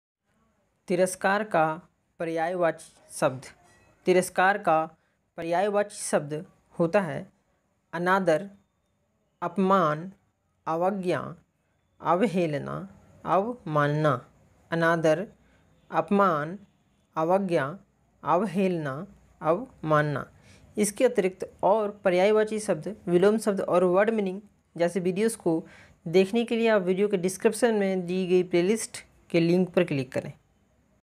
तिरस्कार का पर्यायवाची शब्द । Tiraskar ka paryayvachi shabd। paryayvachi shabd in hindi
Hi

दोस्तों स्वागत है आपका अपने youtube channel  knowledgetalk  में !

दोस्तों आज के इस वीडियो में हमलोग जानेगे की तिरस्कार के पर्यायवाची शब्द क्या है !
तिरस्कार के पर्यायवाची शब्द -
अनादर , अपमान , अवज्ञा , अवहेलना , अवमानना